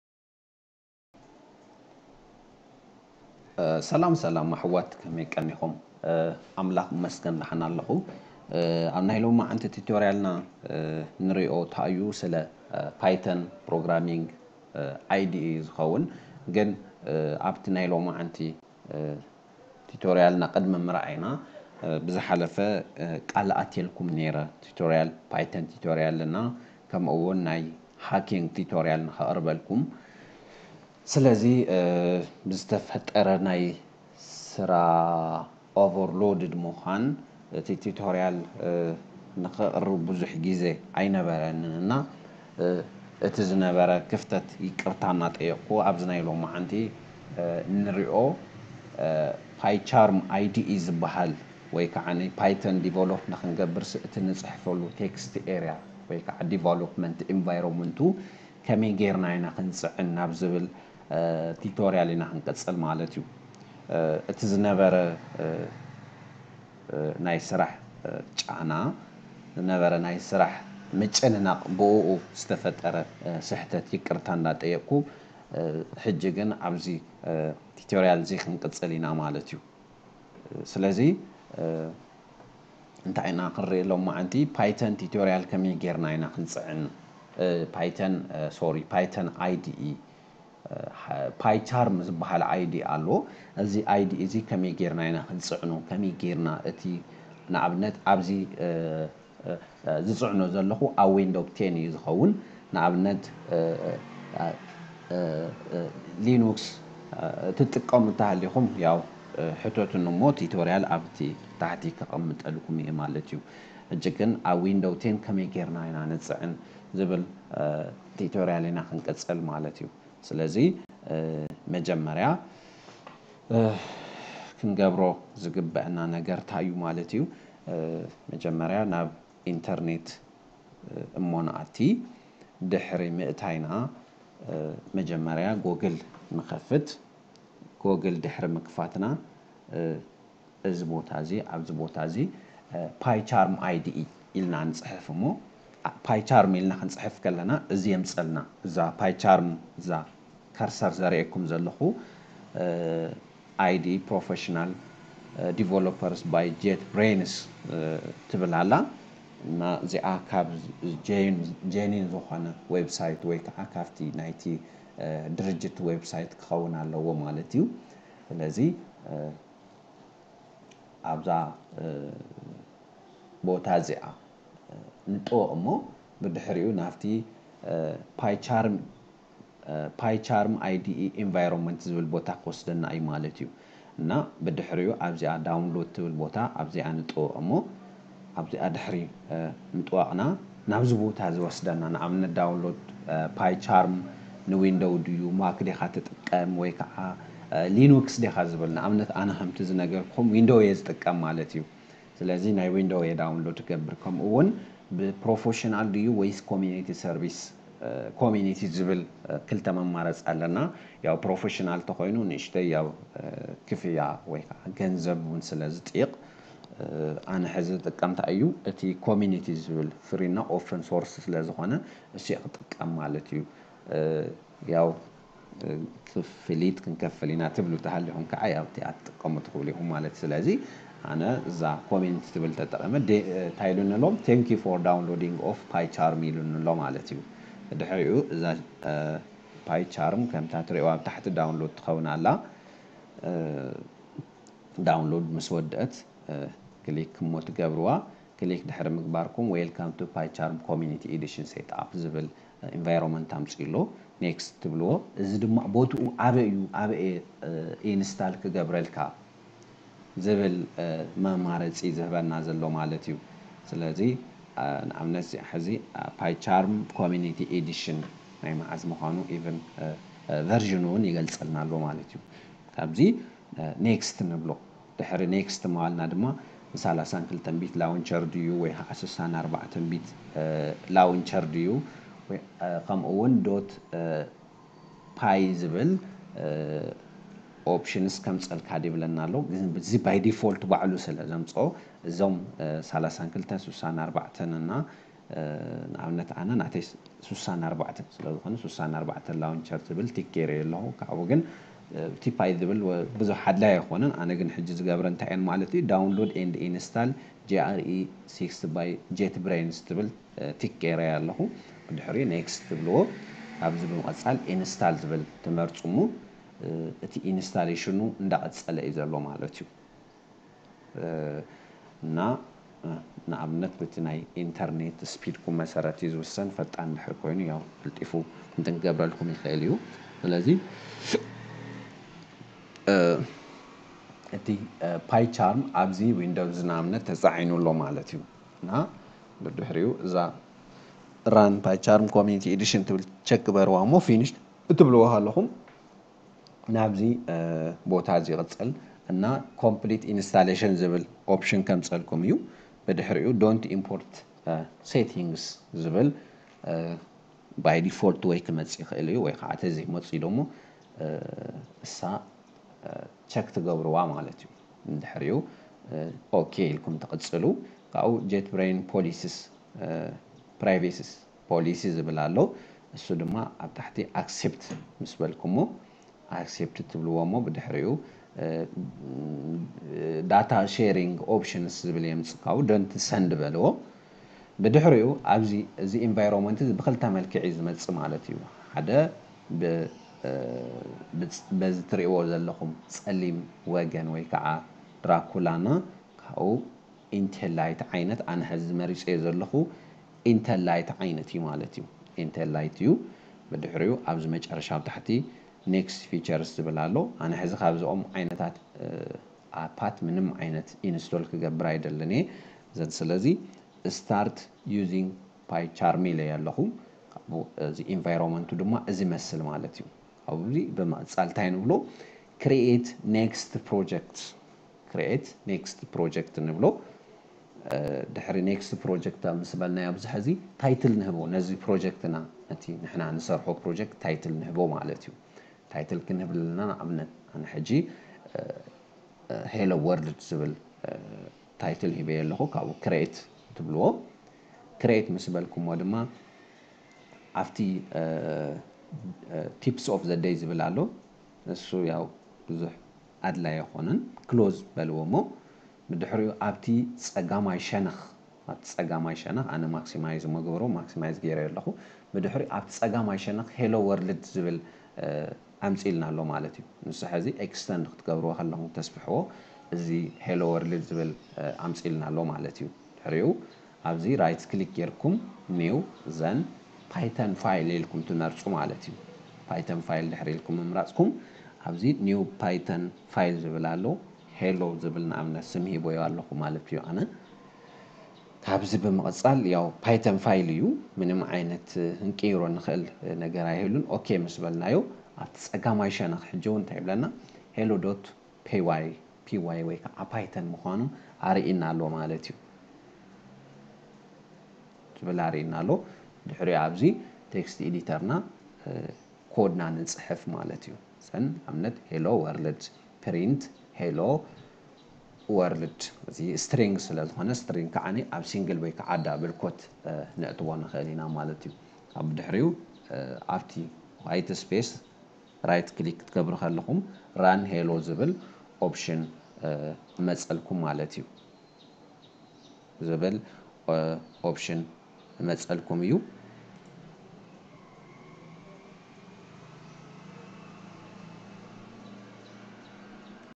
How to Install PyCharm IDE on Windows 10|Pycharm IDE ኣብ Python ከመይ ጌርና ንጽዕን?
1. PyCharm is an integrated development environment (IDE) used in computer programming, specifically for the Python language.